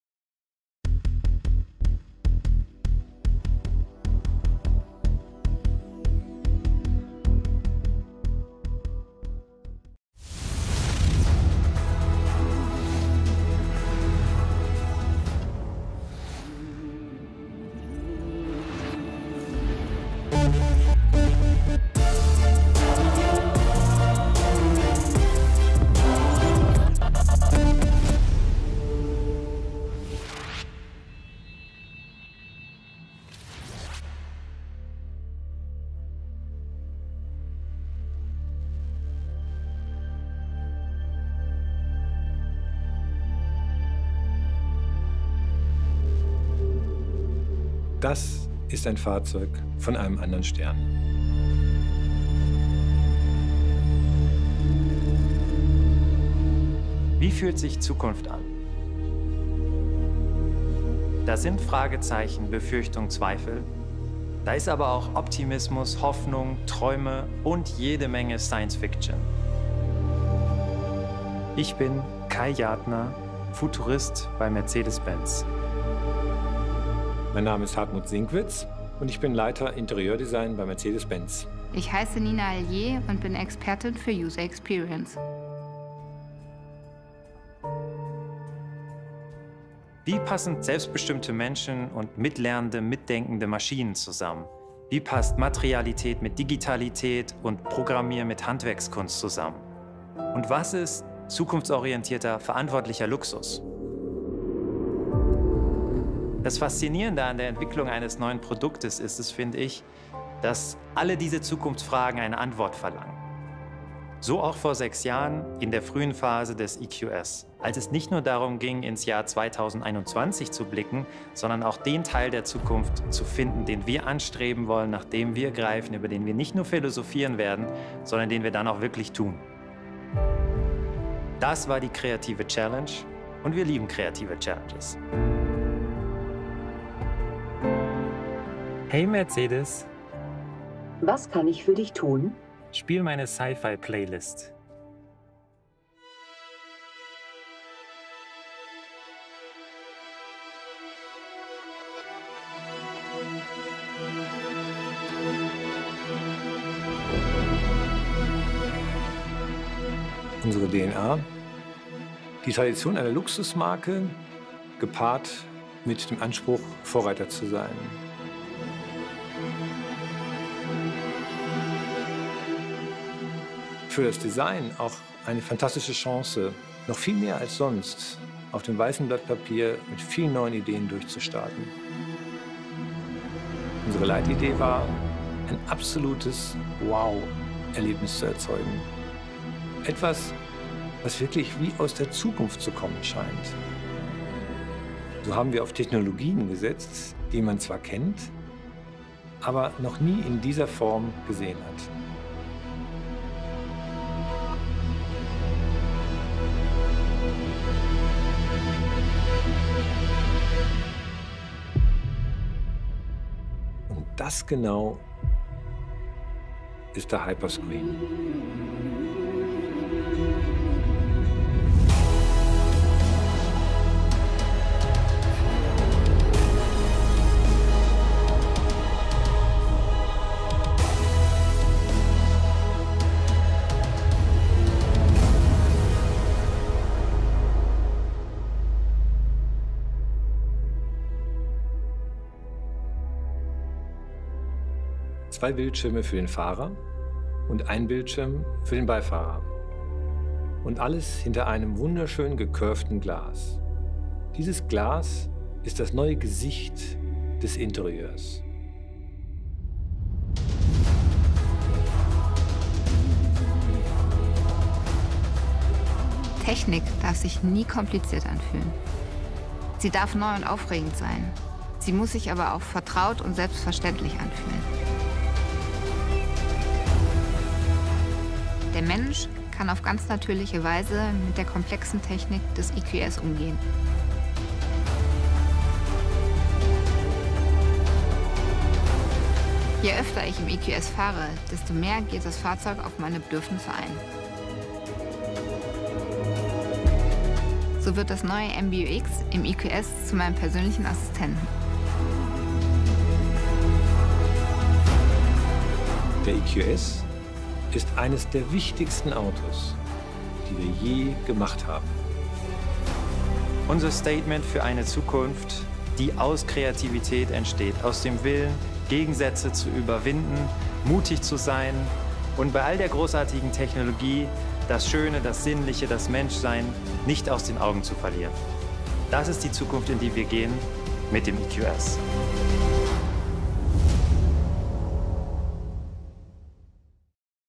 New Mercedes EQS MBUX Infotainment System
This is the new EQS infortainment system, the first all dashboard covered hyperscreen!